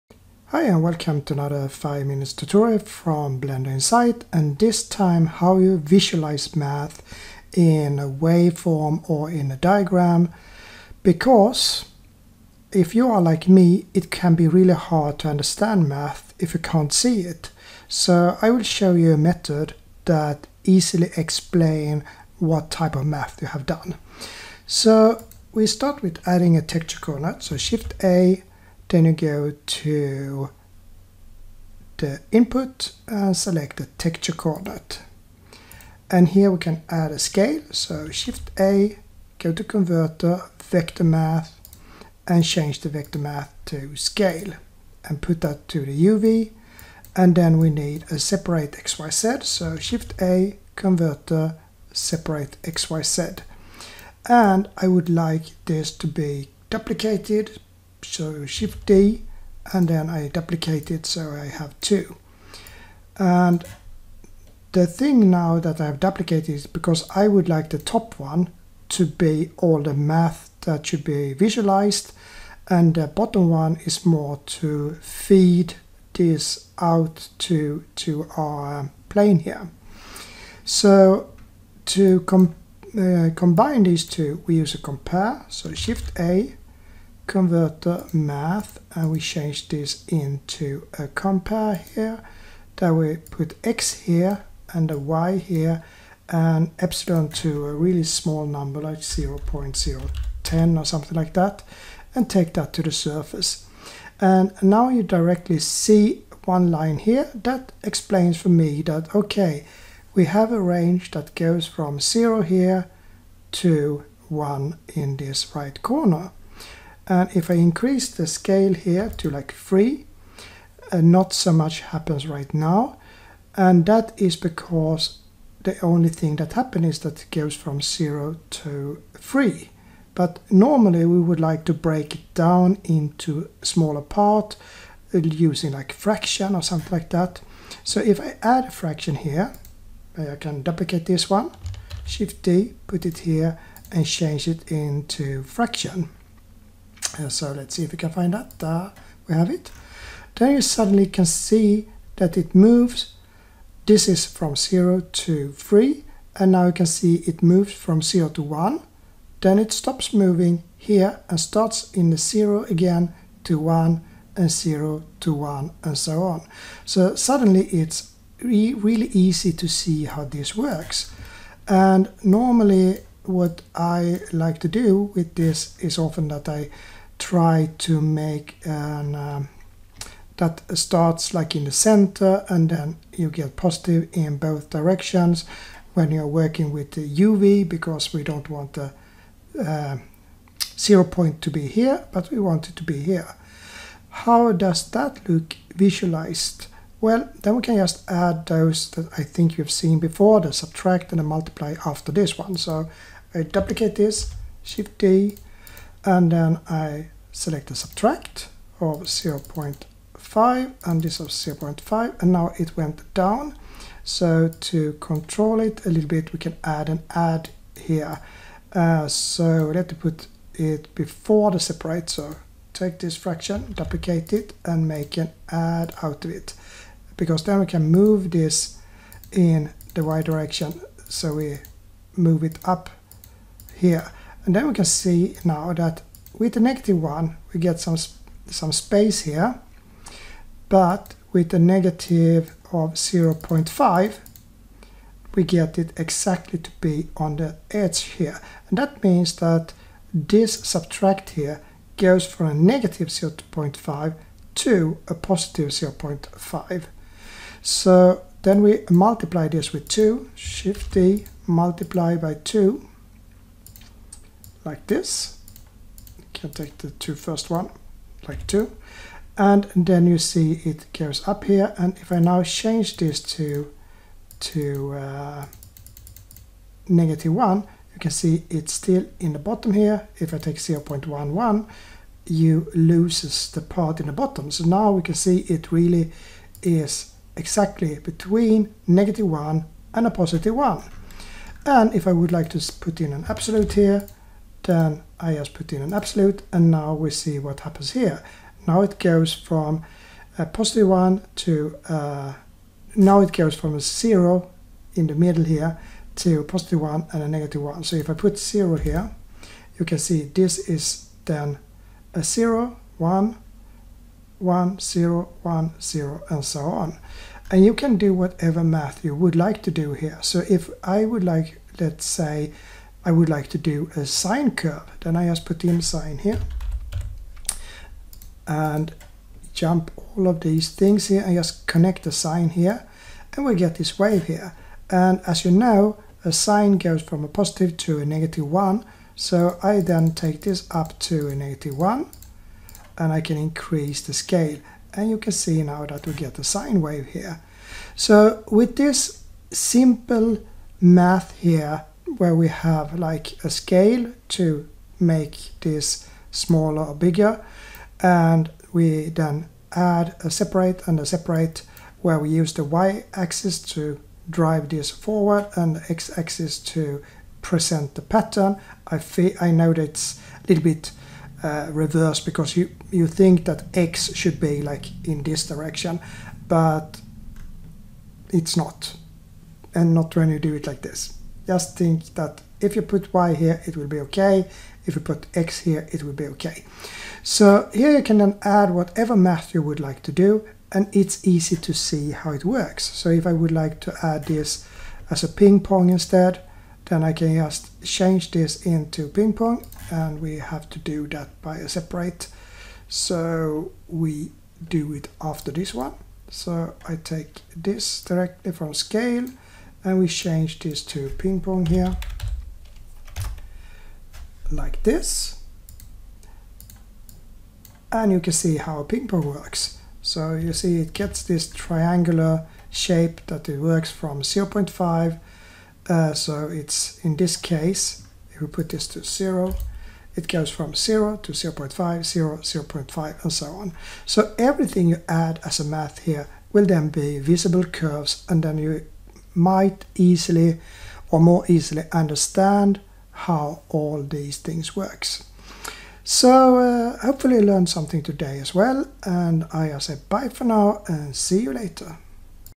83: Visualize math easily!
If you are like me, you might think math is hard to understand. I need to see the result to understand and one way is to make curves from it.
Here is how that works!